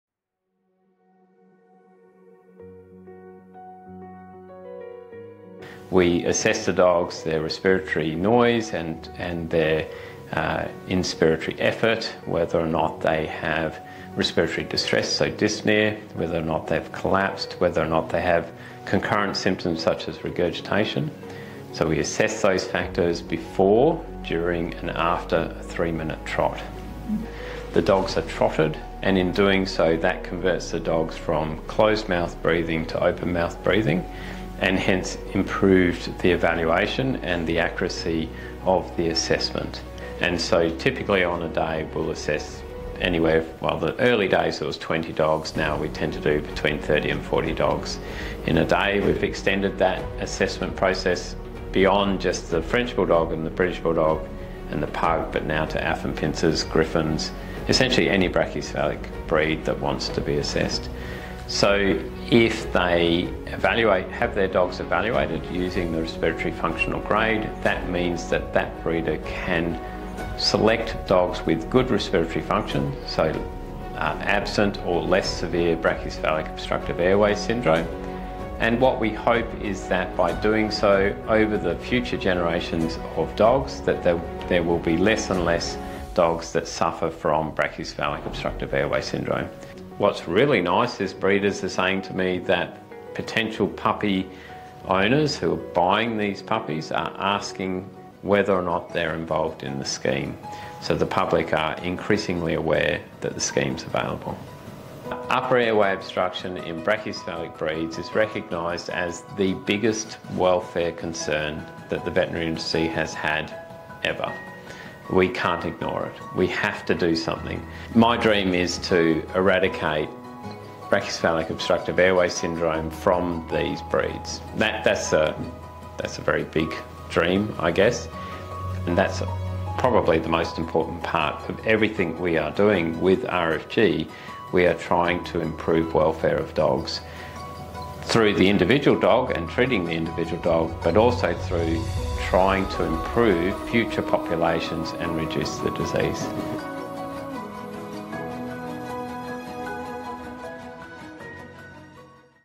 Respiratory function grading for BOAS, with Dr Arthur House, Small Animal Surgery Specialist.
In 2021, with the support of Dogs Australia, Arthur played a pivotal role in bringing the Respiratory Functional Grading (RFG) Scheme to Australia! This video explains what an RFG involves and why it's an imperative step forward for brachycephalic breeds.